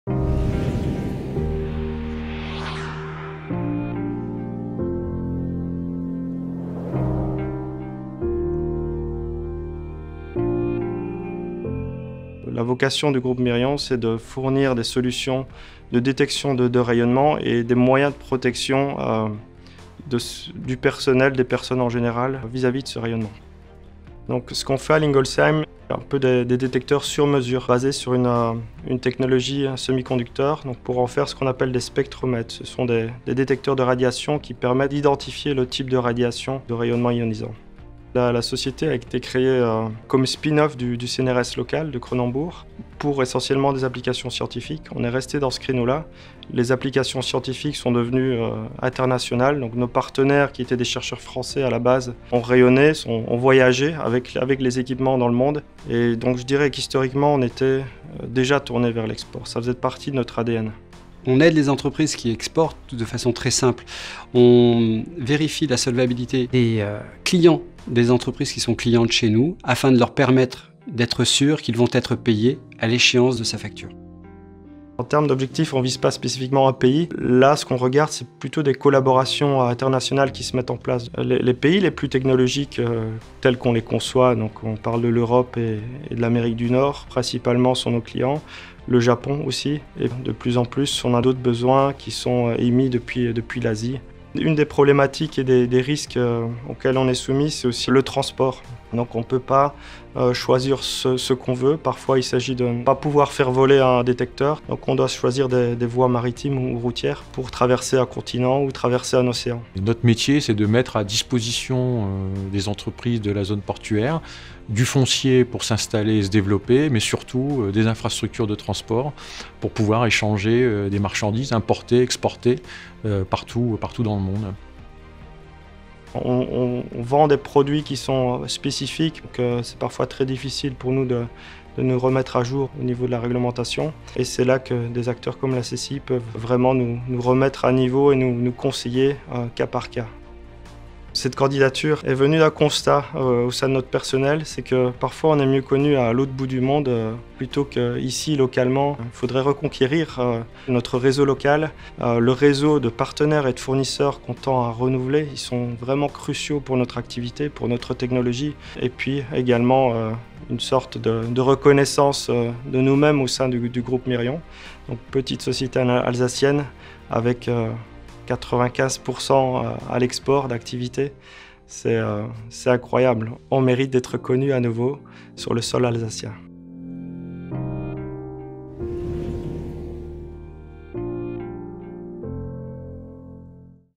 Trophées Alsace Export 2019 - Lauréat catégorie Grand Export : MIRION TECHNOLOGIES
Découvrez l'entreprise MIRION TECHNOLOGIES à Lingolsheim (67), lauréate des Trophées Alsace Export 2019.